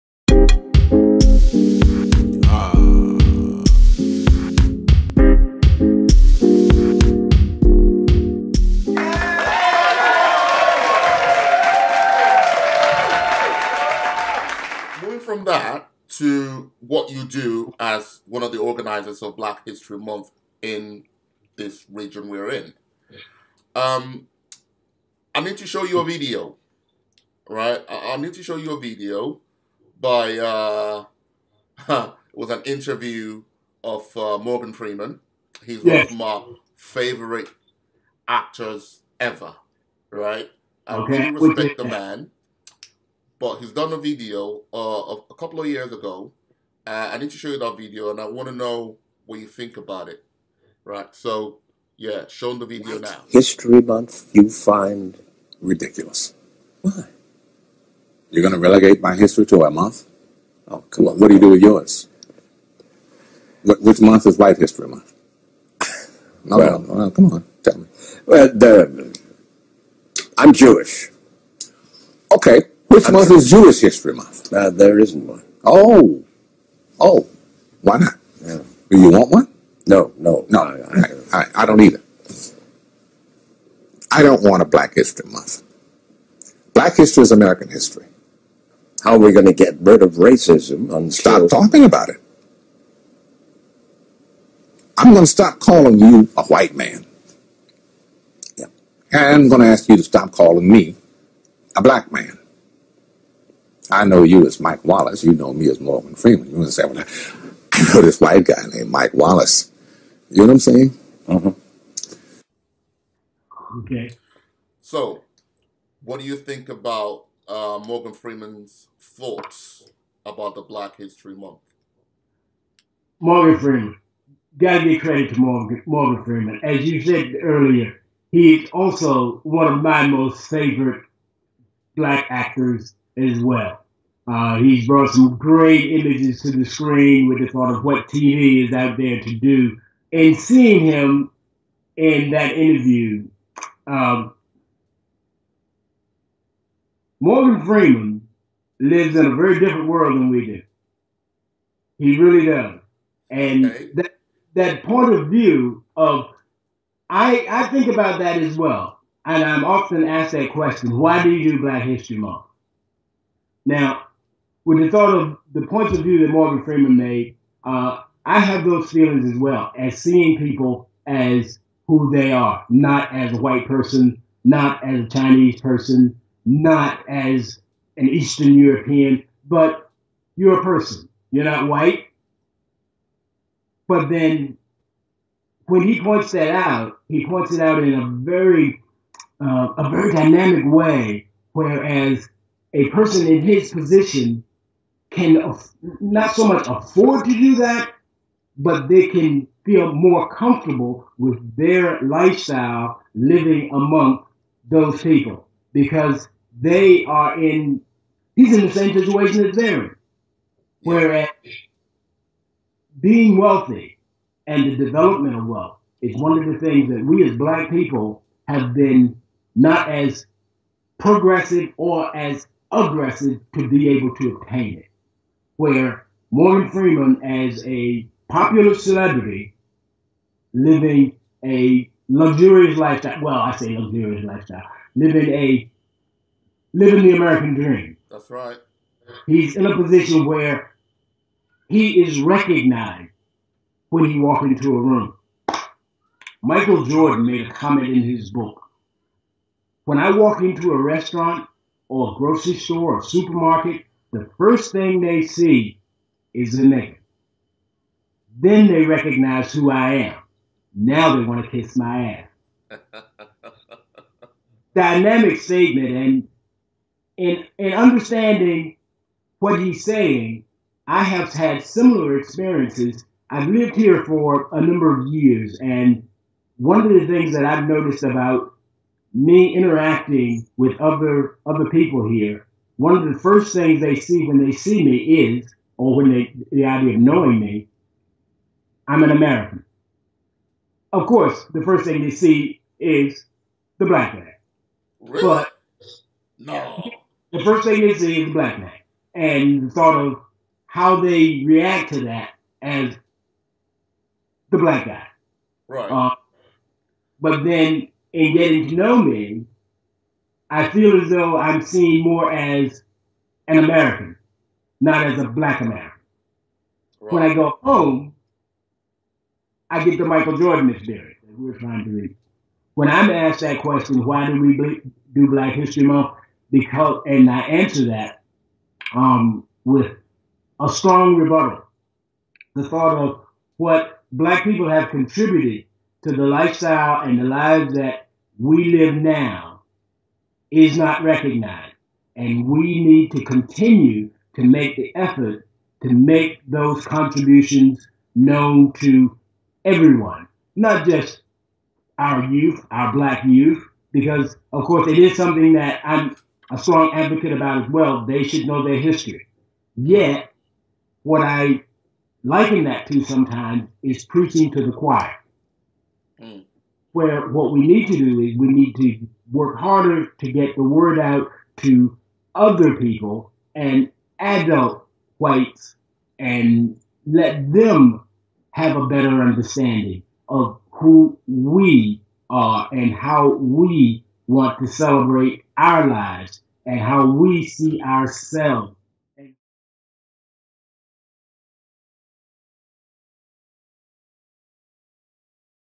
Gist Alert! My guest counters Morgan Freeman's view of Black History Month
Do you agree with celebrating Black History Month? Watch this video to hear what Morgan Freeman and my guest Larry Walker offer their views